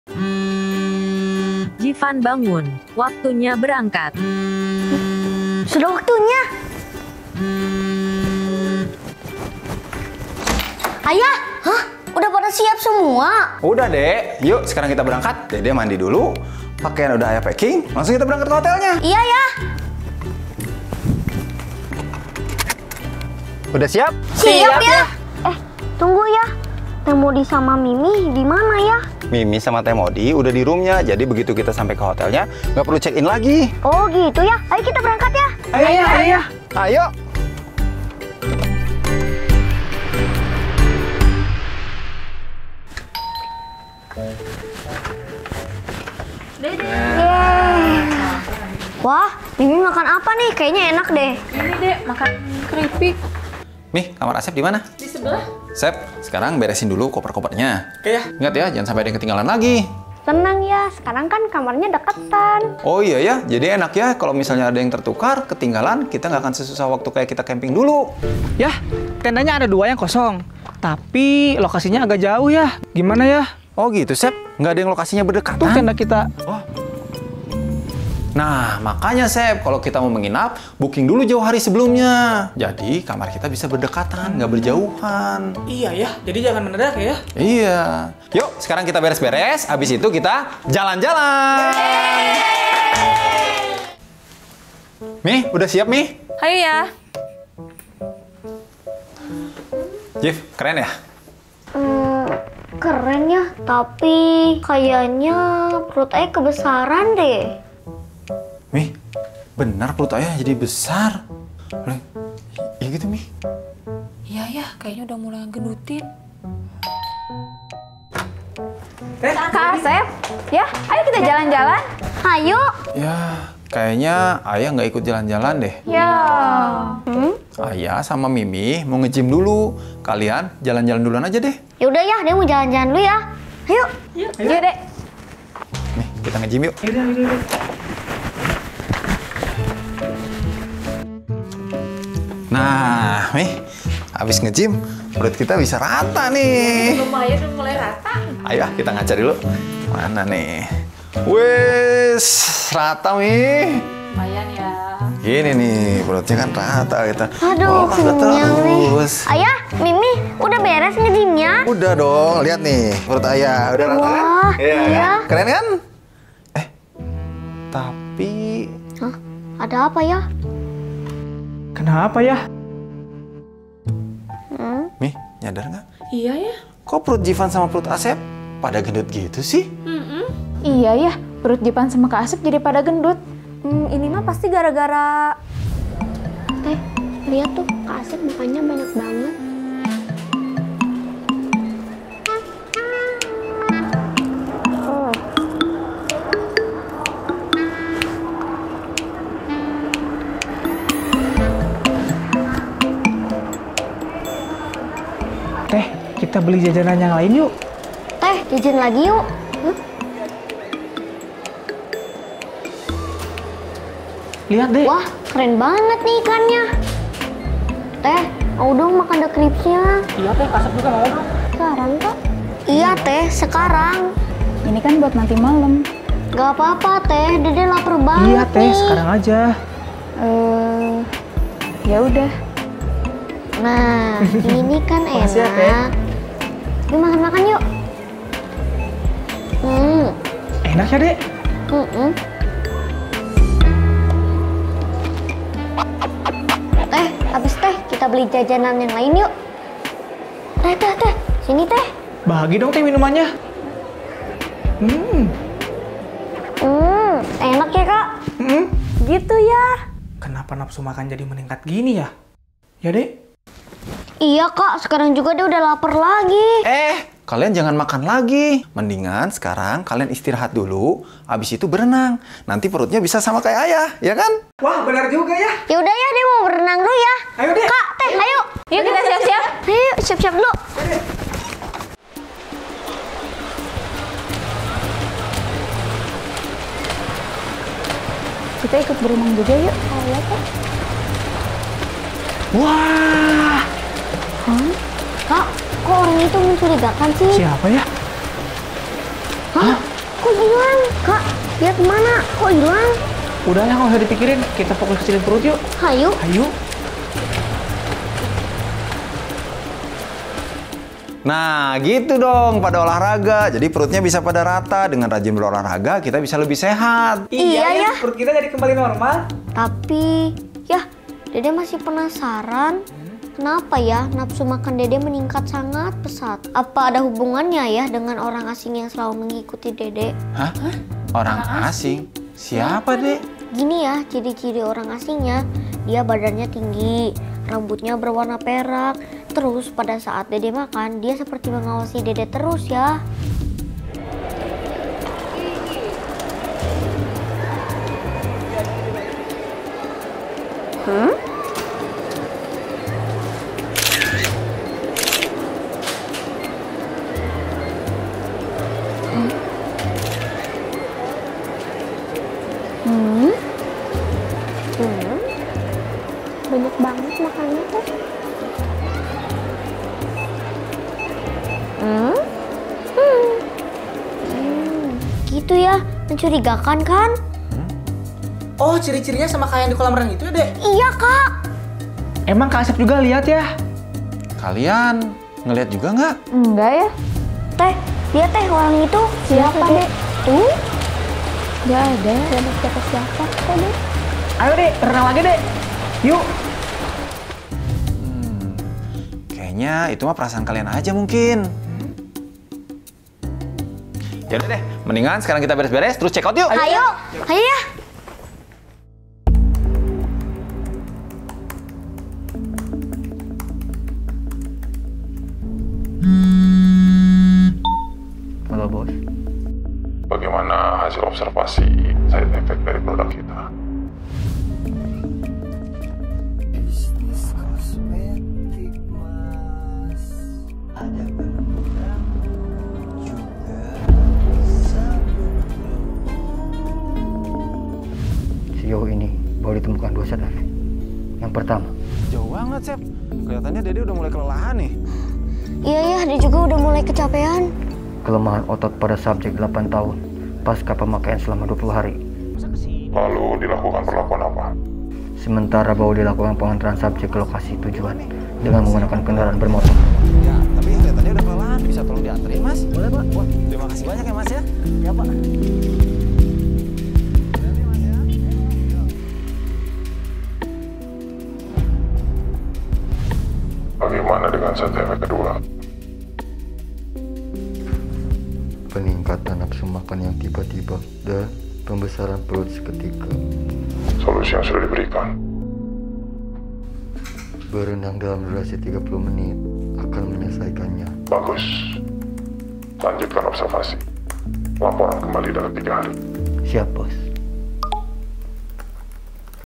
DRAMA | Terungkapnya Misi Pria Berambut Perak Saat Jeevan Menginap di Hotel ⭐5
Menceritakan tentang tujuan pria berambut perak.